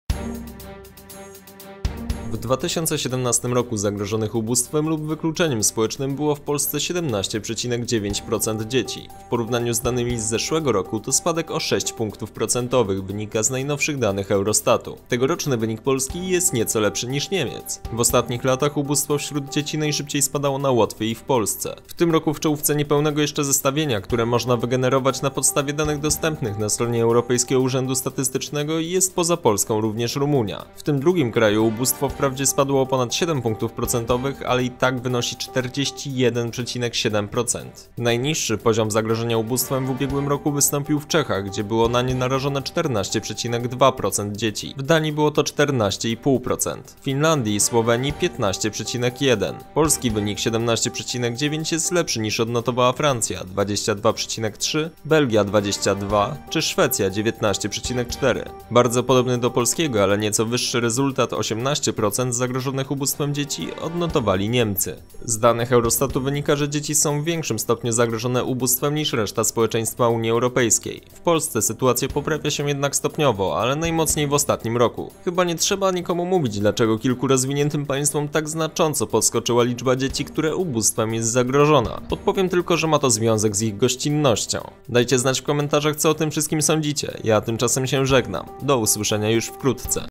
PORAŻKA NIEMIEC! POLSKIE DZIECI MAJĄ LEPSZE WARUNKI DO ŻYCIA | 90 SEKUND
W 2017 r. zagrożonych ubóstwem lub wykluczeniem społecznym było w Polsce 17,9 proc. dzieci. W porównaniu z danymi z zeszłego roku to spadek o 6 pkt proc. - wynika z najnowszych danych Eurostatu. Tegoroczny wynik Polski jest nieco lepszy niż Niemiec.

W ostatnich latach ubóstwo wśród dzieci najszybciej spadało na Łotwie i w Polsce. W tym roku w czołówce niepełnego jeszcze zestawienia (nie ma danych wszystkich krajów), które można wygenerować na podstawie danych dostępnych na stronie europejskiego urzędu statystycznego, jest poza Polską również Rumunia. W tym drugim kraju ubóstwo wprawdzie spadło o ponad 7 pkt proc., ale i tak wynosi 41,7 proc.

Najniższy poziom zagrożenia ubóstwem w ubiegłym roku wystąpił w Czechach, gdzie było na nie narażone 14,2 proc. dzieci. W Danii było to 14,5 proc., w Finlandii i Słowenii - 15,1 proc.

Z danych Eurostatu wynika, że dzieci są w większym stopniu zagrożone ubóstwem niż reszta społeczeństwa UE. W Polsce sytuacja poprawia się jednak stopniowo, ale najmocniej w ostatnim roku. Na przykład w 2008 r. problem biedy i wykluczenia dotykał ok. 33 proc. dzieci, w 2012 r. - 29,3 proc., w 2015 r. było to 26,6 proc., a w 2016 r. - 24,2 proc.

Chyba nie trzeba nikomu mówić dlaczego kilku rozwiniętym państwom tak znacząco podskoczyła liczba dzieci, które ubóstwem jest zagrożona. Podpowiem tylko, że ma to związek z ich gościną. Dajcie znać w komentarzach co o tym wszystkim sądzicie, ja tymczasem się żegnam. Do usłyszenia już wkrótce.

Lektor: Filip Kupczyński